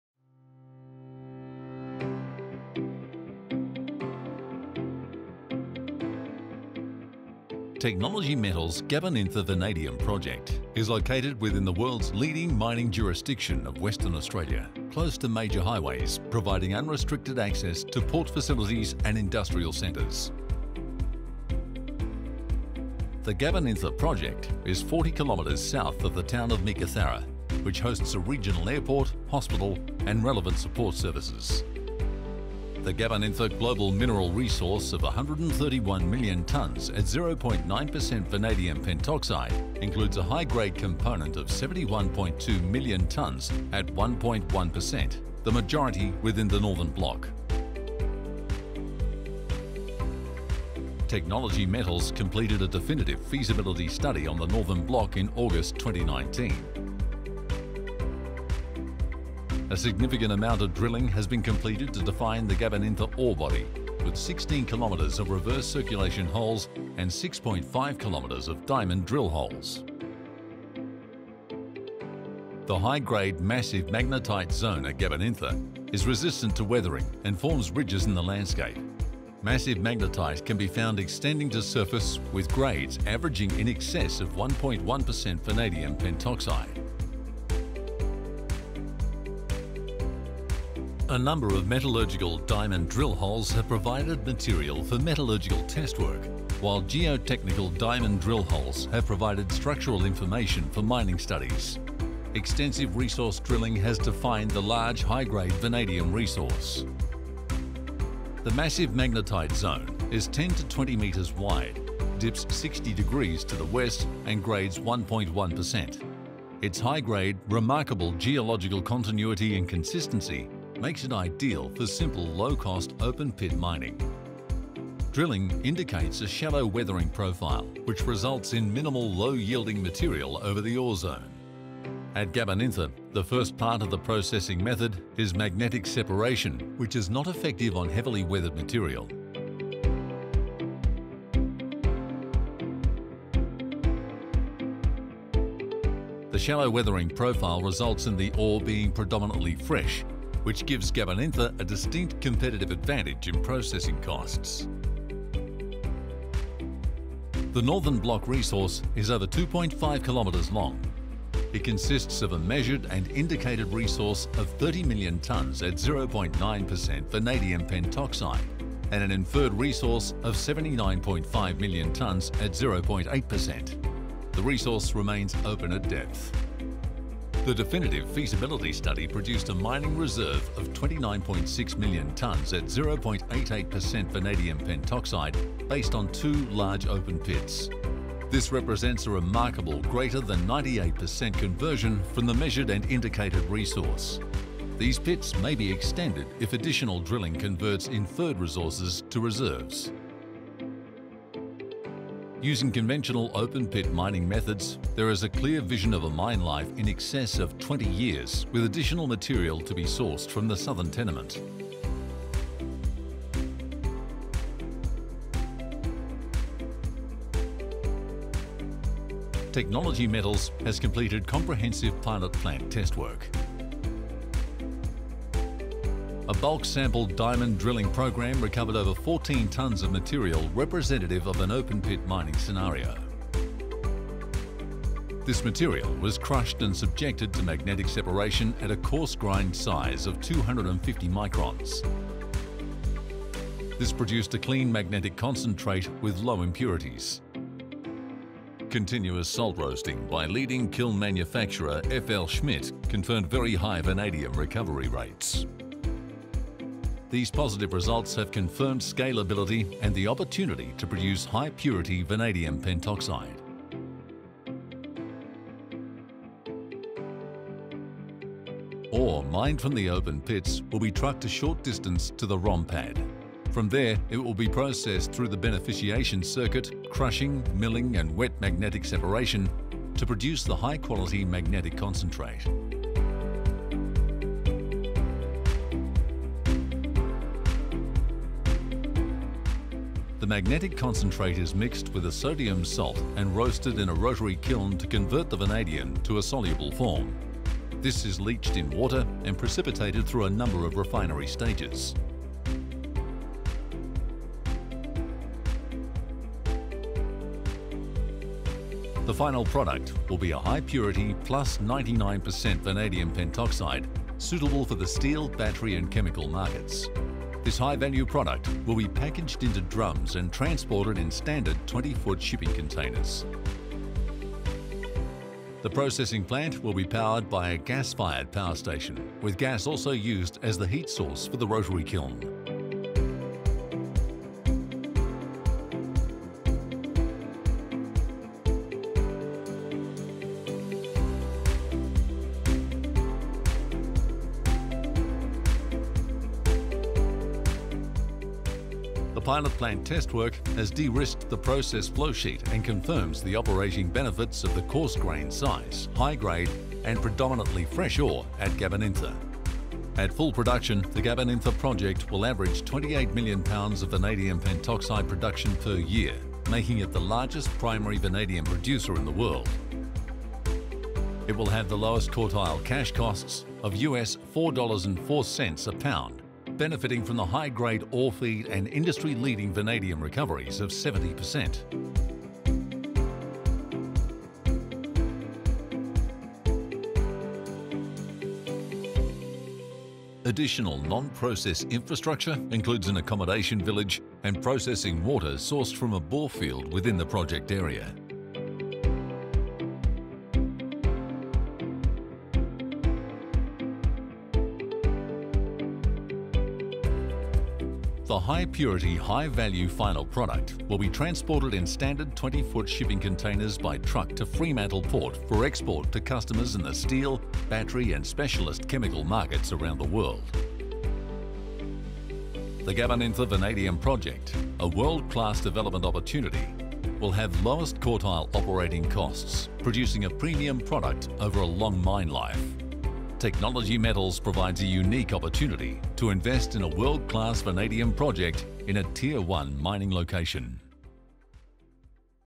Gabanintha Vanadium Project Fly Through
Technology Metals Australia Limited (ASX: TMT) is focused on developing its 100% owned flagship Gabanintha Vanadium Project located 40km south of Meekatharra in Western Australia. Once operational, Gabanintha will be the largest pure play vanaidum mine in the world and operating at lowest quartile cash costs. With a clear vision of a greater than 20 year mine life, Gabanintha represents a tier one vanadium project. Recent binding offtakes and MOUs demonstrate the scale and quality of the project.